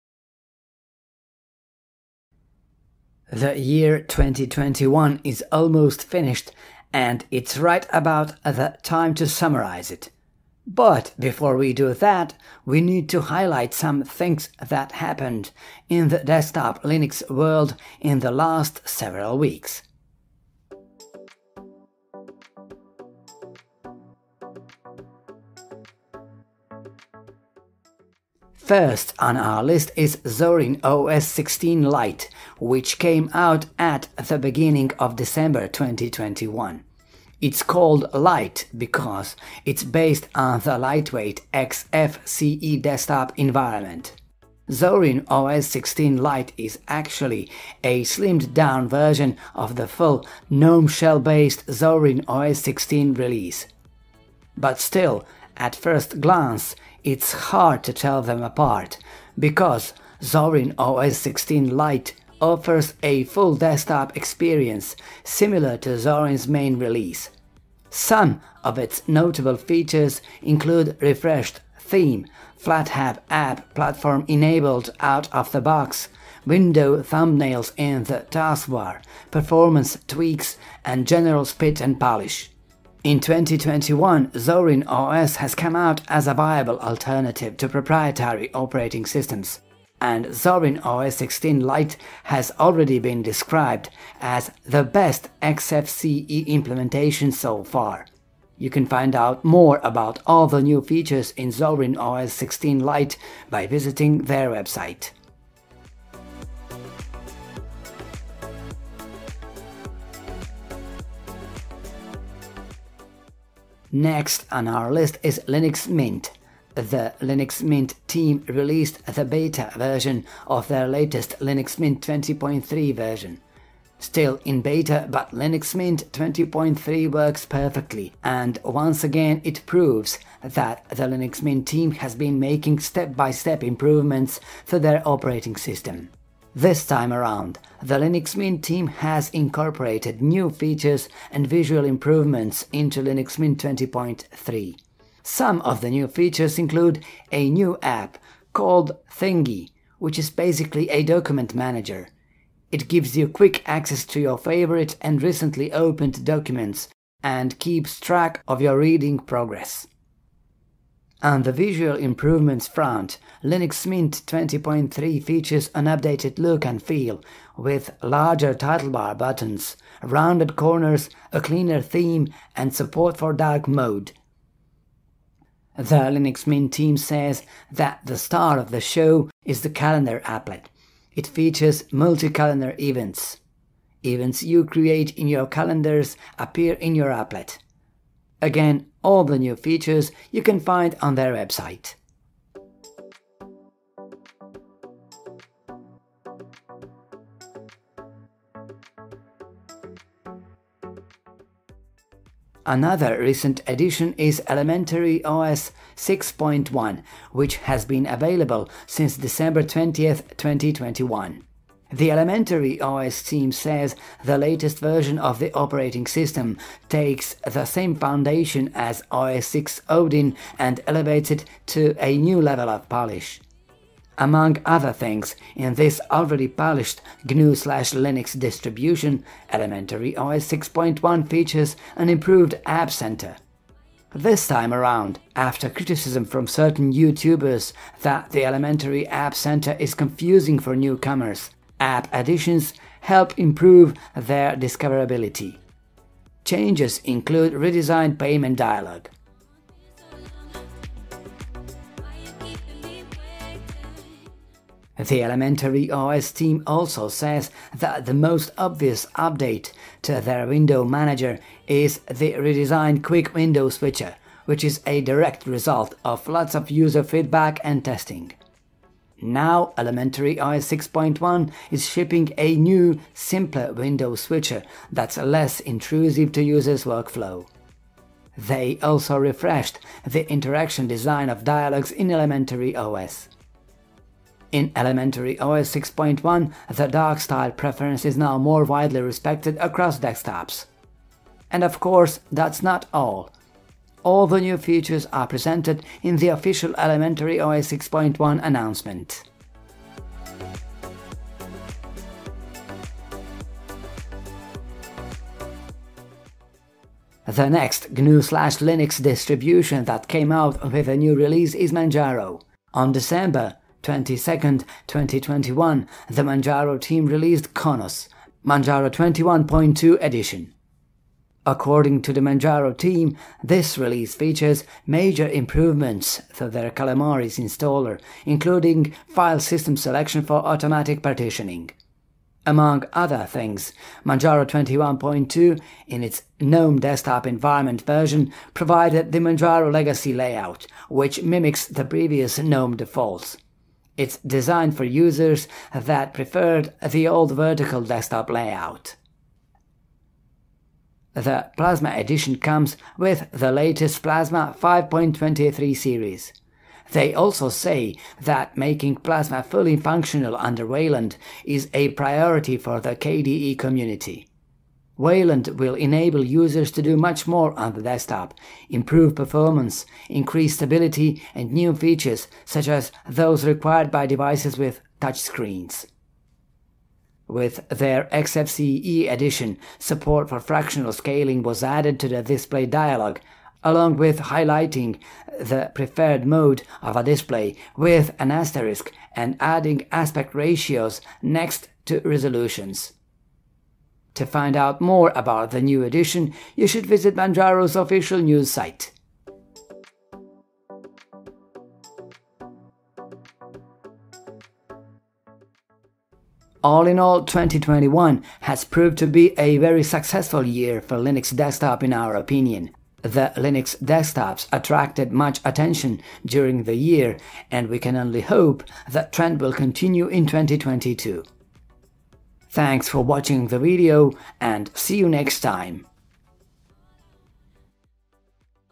What's new in Zorin OS, Linux Mint, elementary OS and Manjaro?
What an end of the year 2021!

The year 2021 is almost finished, and it’s right about the time to summarize it. But, before we do that, we need to highlight some things that happened in the desktop Linux world in the last several weeks...